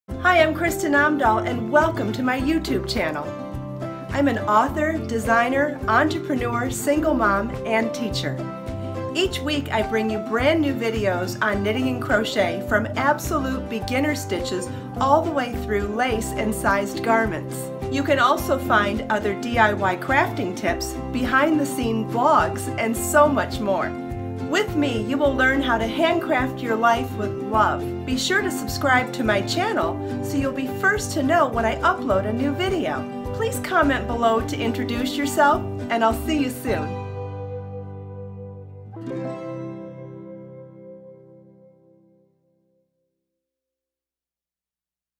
Kristin Omdahl YouTube Channel Trailer Knitting Crochet Craft DIY Handcraft Your Life with Love
Kristin Omdahl Knitting Crochet Craft DIY YouTube Channel Trailer Handcraft Your Life with Love
I’m an author, designer, entrepreneur, single mom and teacher. Each week I bring you brand new videos on knitting and crochet, from absolute beginner stitches to lace and sized garments! You can also find other DIY crafting tips, behind-the-scenes vlogs and so much more! With me, you will learn how to hand craft your life with love.

eucalan.com for Wrapture all natural delicate wash